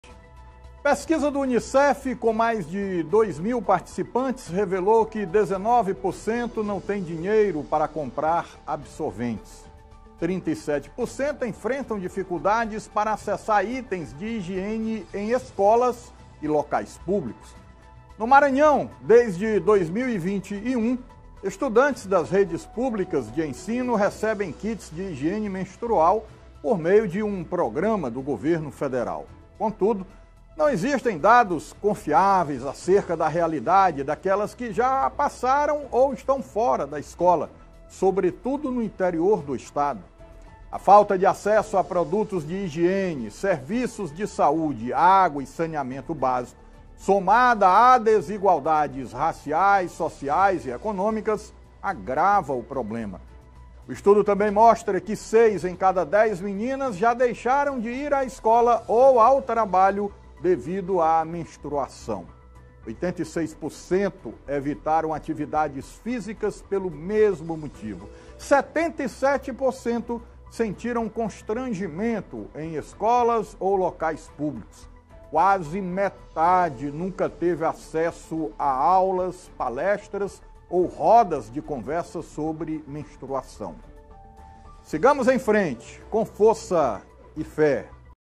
Seis em cada dez meninas no Brasil já deixaram de ir à escola ou ao trabalho devido à menstruação
A falta de acesso a produtos de higiene, serviços de saúde, água e saneamento básico, somada a desigualdades raciais, sociais e econômicas, agrava o problema